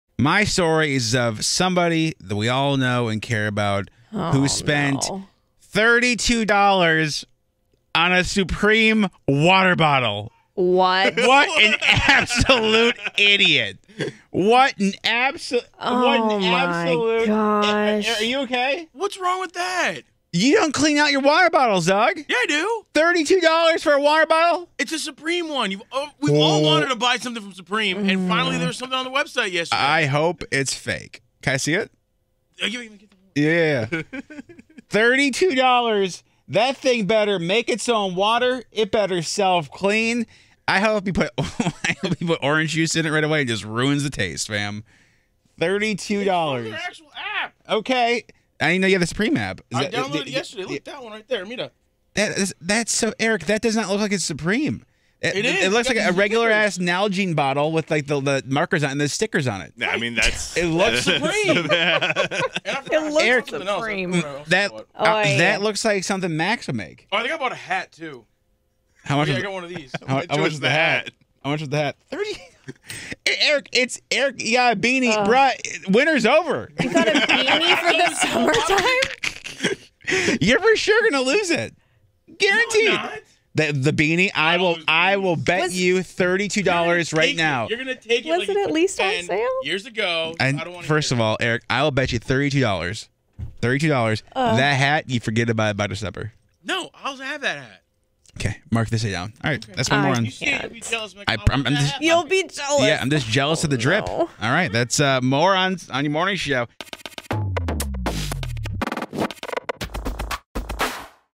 Erick Spent How Much On A Supreme Water Bottle?? | YMS
How fast do you think Erick is going to lose his Supreme water bottle and beanie that he spent $70 on??